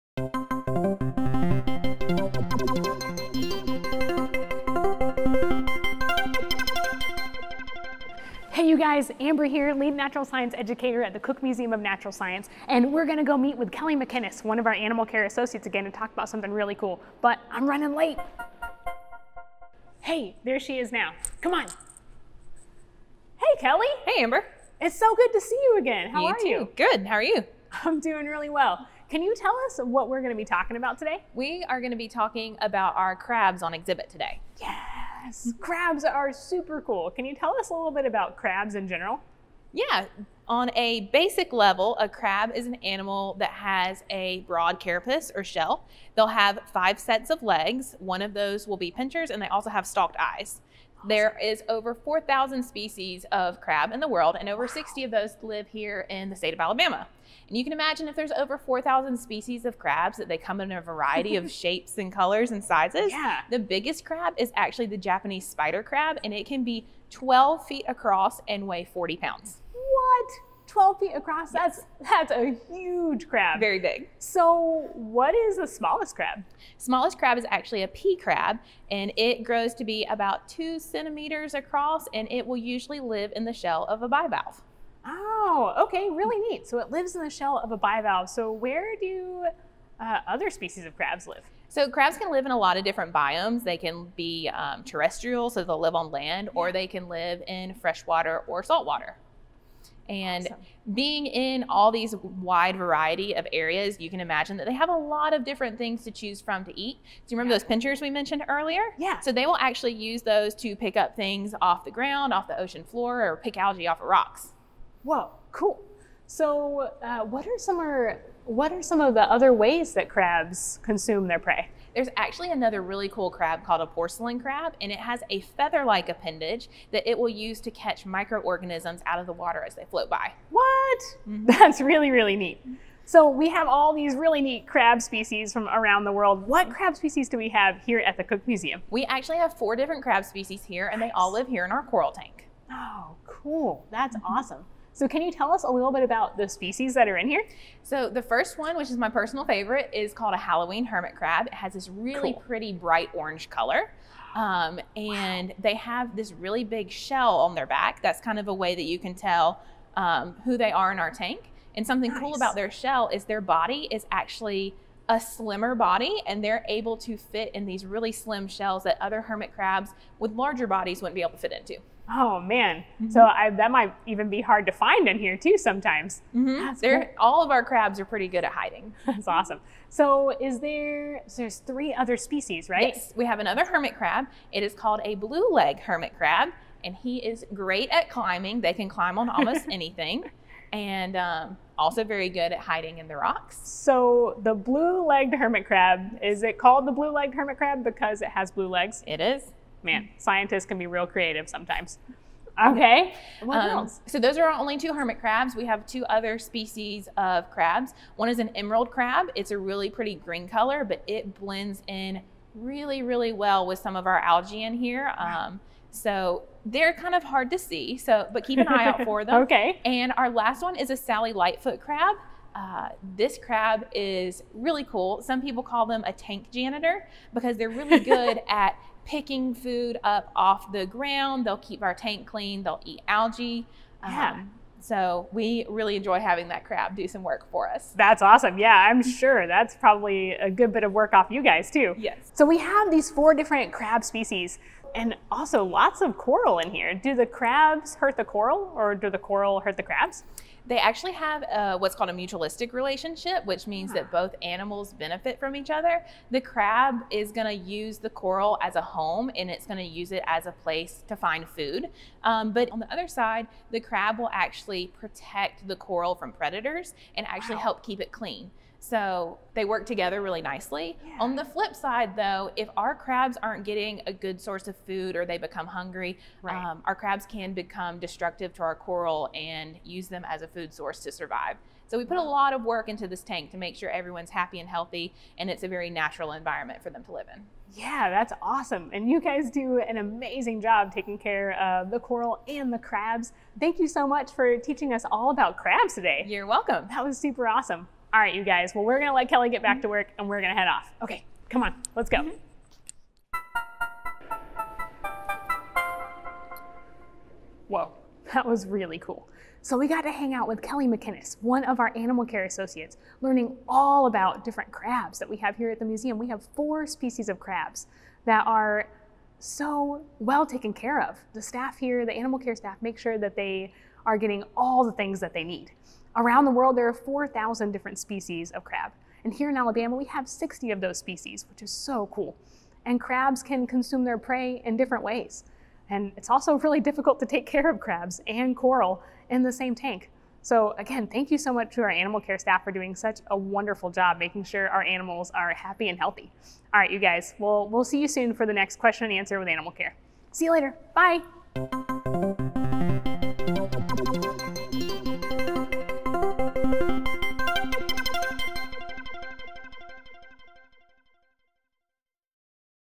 Q&A with Animal Care: Crabs
There are 4,000 species of crabs around the world and the state of Alabama is home to 60 of those! Today we are learning all about crabs and the 4 species we have in our Coral tank.